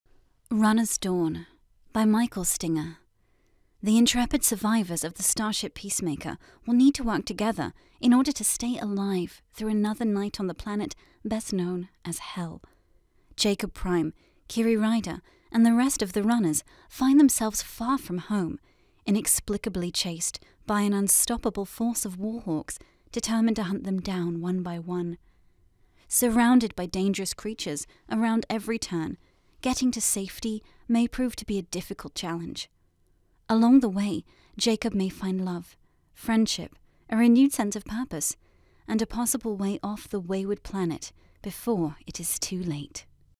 Runner's Dawn - By Michael Stinger (Concept Trailer)
* Also included in the brand new set:  "Runner's Chronicles - Origins: Volume 1"

Runner's Dawn - By Michael Stinger

The intrepid survivors of the starship Peacemaker will need to work together in order to stay alive through another night on the planet best known as 'Hell'.

Jacob Prime, Kiri Ryder, and the rest of the Runners find themselves far from home; inexplicably chased by an unstoppable force of Warhawks determined to hunt them down one by one.

Surrounded by dangerous creatures around every turn, getting to safety may prove to be a difficult challenge.

Along the way, Jacob may find love, friendship, a renewed sense of purpose, and a possible way off the wayward planet before it is too late...

(Available in eBook, Paperback and Hard Cover)